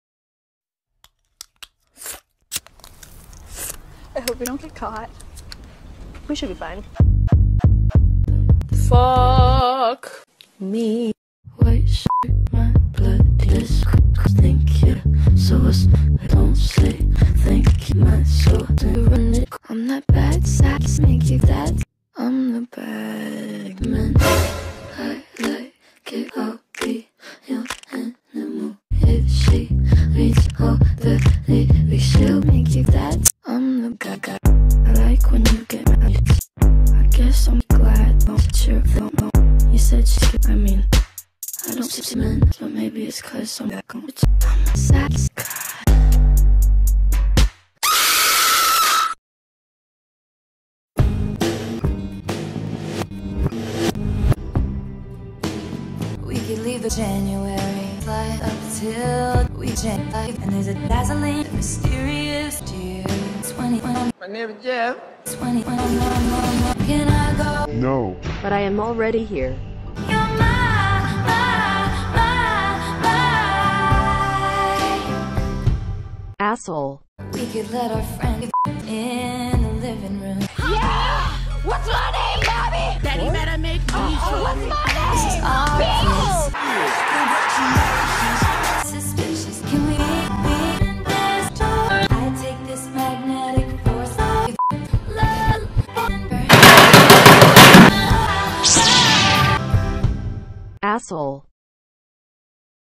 YTP- Billie Eilish becomes Batman while Taylor Swift kills her bullies with a lightsaber
a request from Mandarin Oranges and Pizza/French Fries and Cheeseburgers/Swear to be overdramatic and true to my lover.